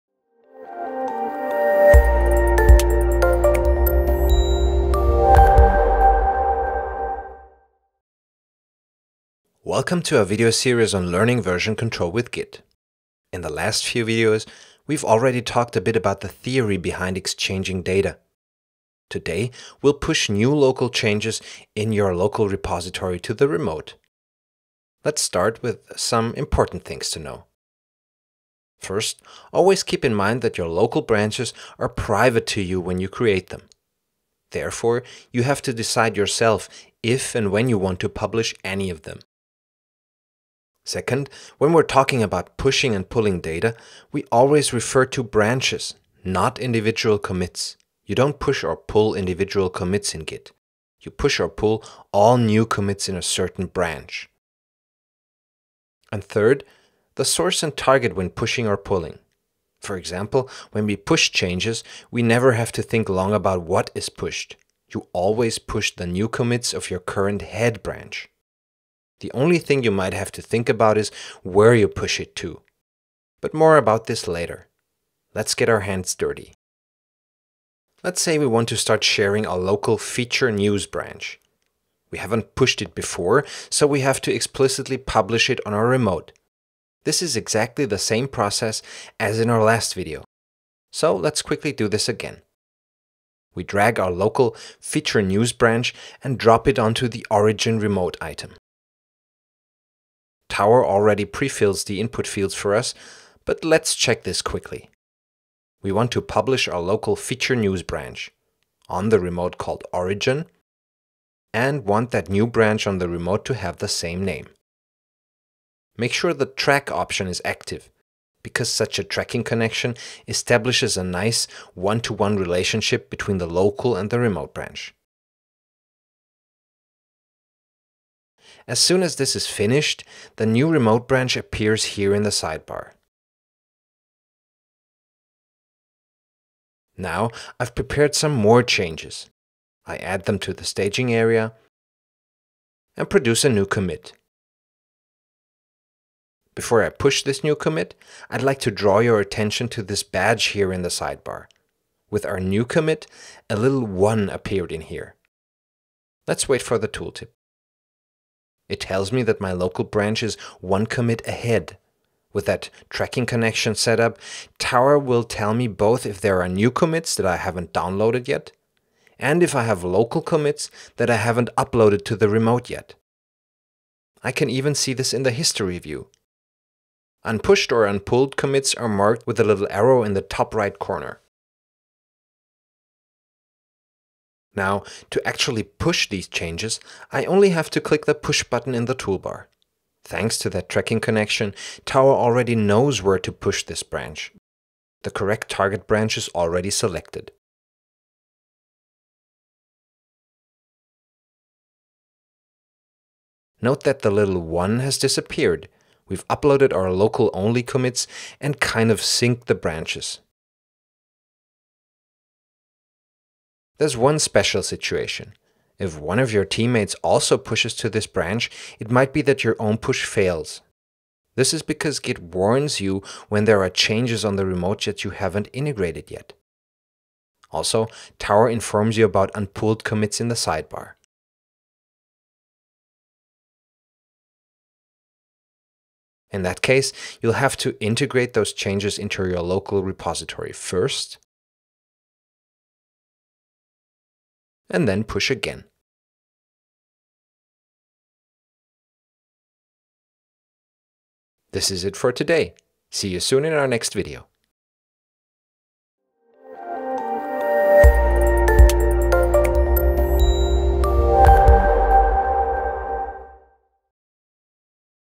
Pushing Changes to a Remote [Learn Git Video Course]
How can I push my changes to the remote server? What are tracking connections?